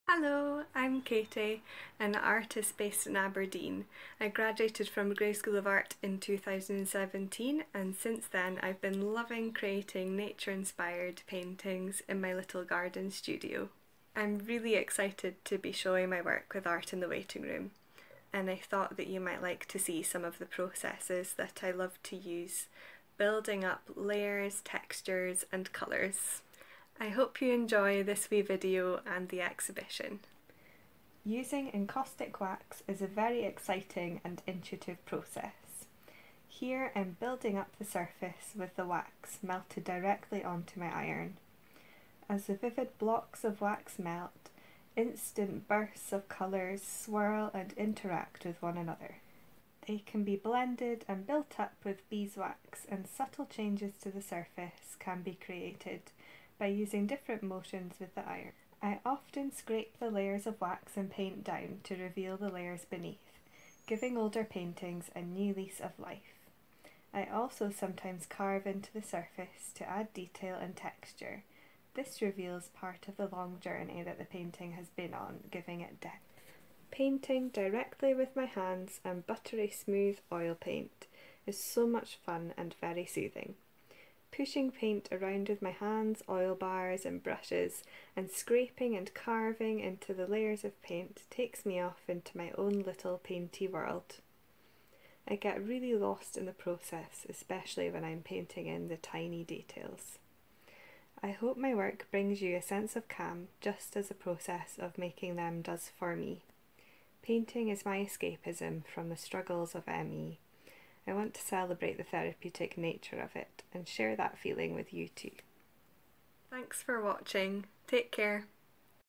Art in the Waiting Room - Katie Hammond Virtual Solo Exhibition 2021
An introduction to my virtual solo exhibition with Art in the Waiting Room and a little look at some of the processes I love when making my paintings.

You can also join us on Facebook: @artinthewaitingroom

25% of all sales will go directly to support Camphill and their various projects. We hope you can join us from the comfort of your home :)